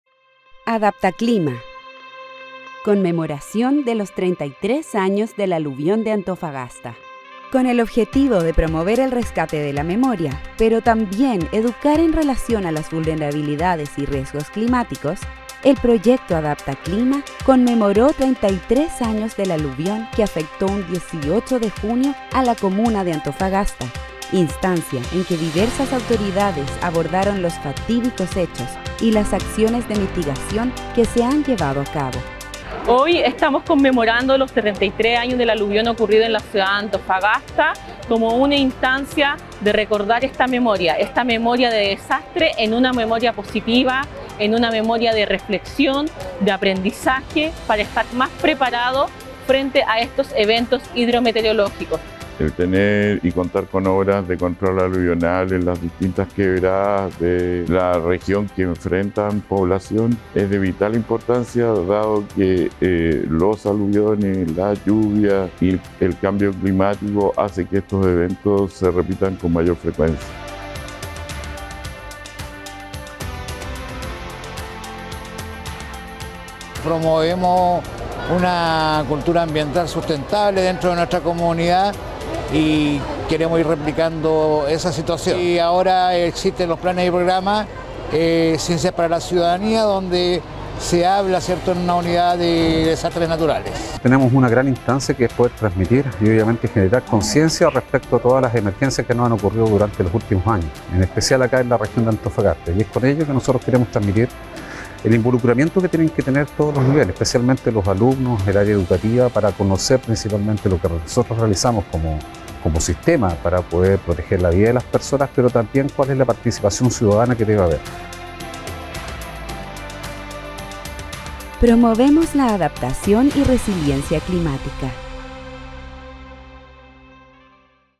Antofagasta | Conmemoración 33 años del aluvión
En honor al 33° aniversario del devastador aluvión de Antofagasta, el proyecto AdaptaClima organizó una serie de actividades destinadas a educar, empoderar y transformar comunidades vulnerables en comunidades resilientes frente a los riesgos climáticos.